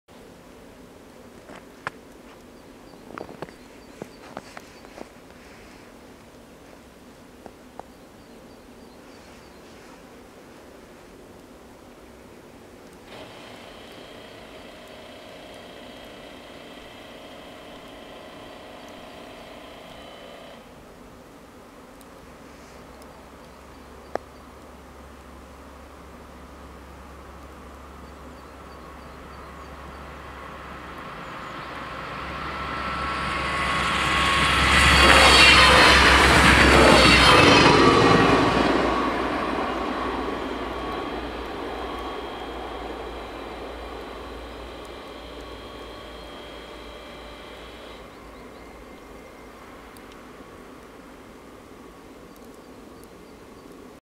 Spoorwegovergang Marsberg (D) // Railroad crossing // Bahnübergang
Locatie: L870
Traject: Brilon Wald - Warburg
Rode lichten: 4
Bomen: 2
Andreaskruisen: 4
Passeren:
- Talent als RE17 Kassel-Wilhelmshöhe → Hagen